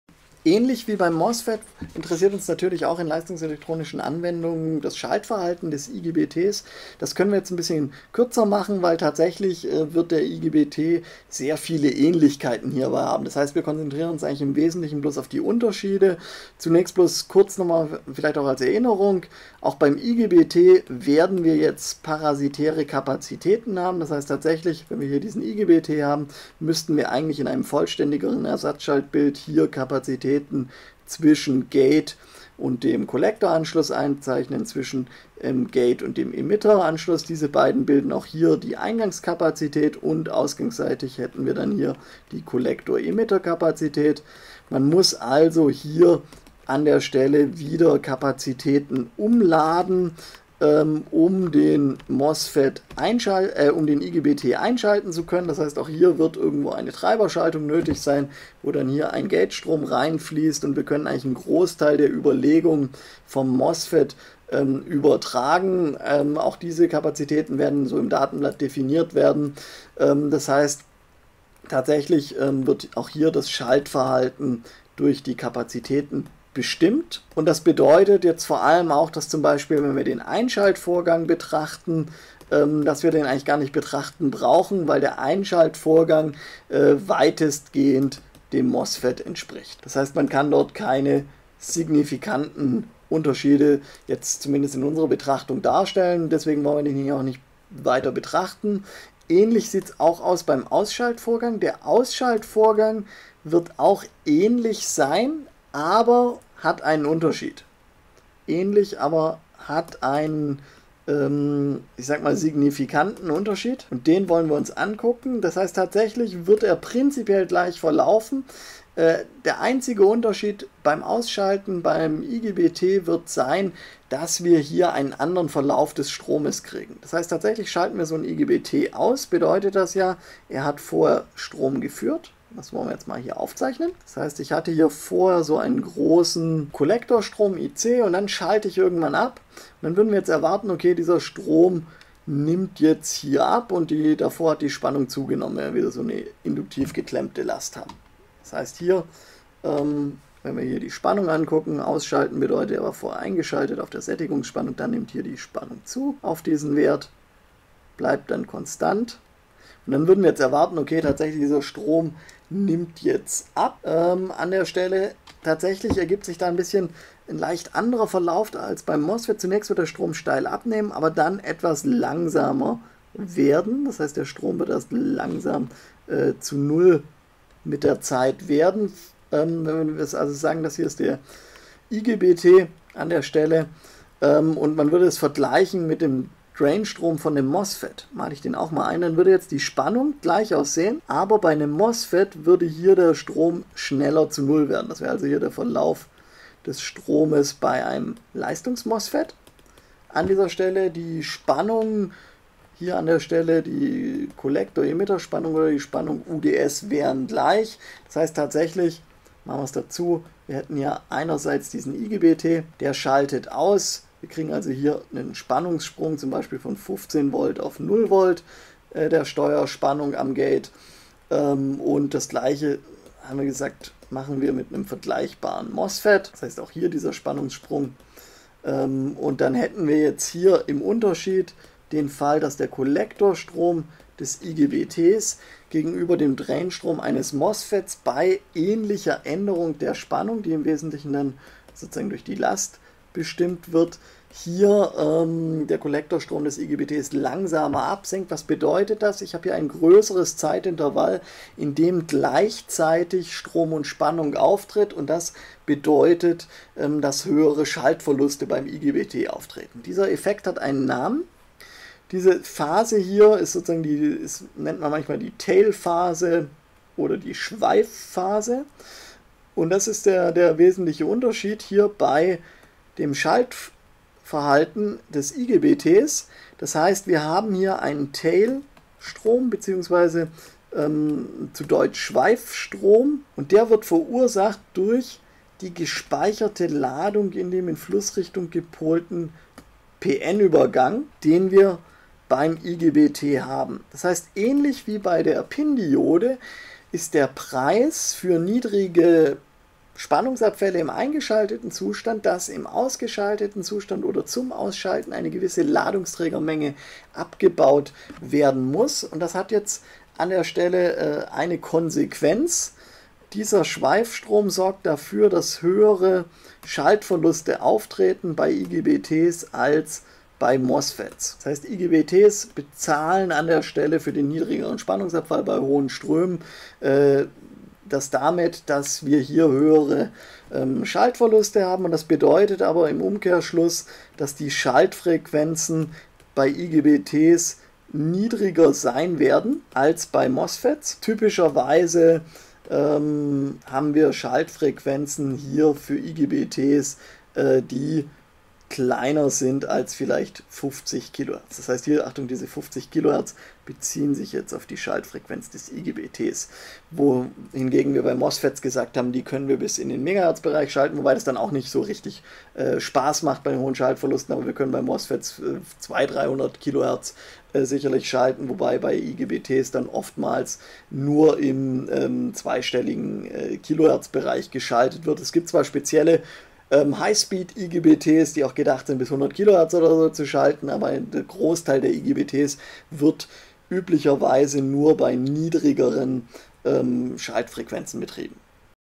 LE3_5e IGBT Dynamisches Schaltverhalten
Vorlesung Leistungselektronik im Studiengang Mechatronik an der DHBW Stuttgart Campus Horb.
In diesem Teil wird auf das Schaltverhalten des IGBT im Vergleich zum Leistungsmosfet eingegangen.